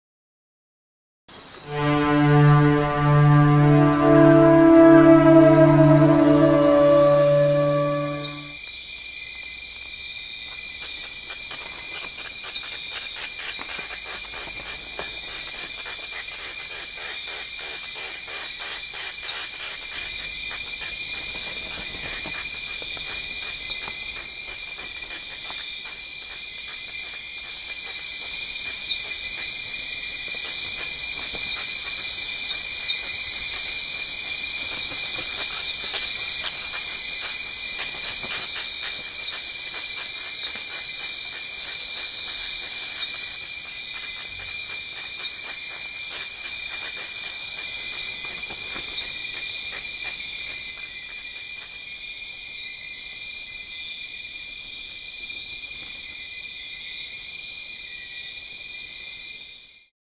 The Oilbird Uses Echolocation
The Oilbird if the world's only night flying bird that eats fruit. It roosts in colonies in caves and dark ravines and like bats it uses echolocation to navigate. Oilbirds love to eat palm nuts and small wild avocados. While flying they make audible clicking noises.